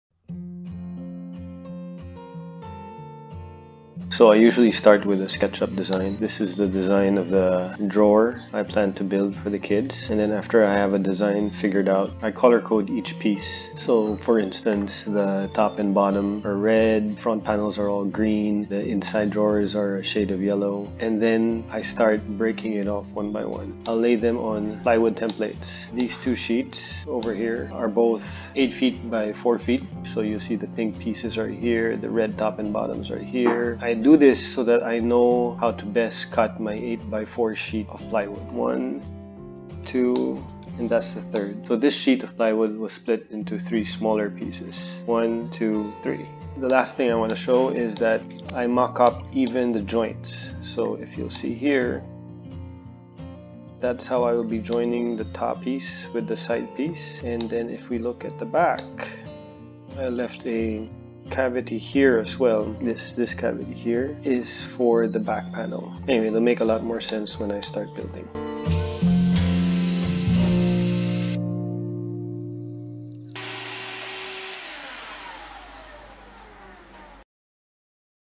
Drawer Build Part 01 Sketchup
This is part 1 of the Drawer Build Project. Here I show the app I use for most, if not all, of my projects. Sketchup is a simple tool to design and visualize a build and best of all - it's free! I use this app to determine how many pieces of plywood I need to buy and from there plot out the parts I need for the build.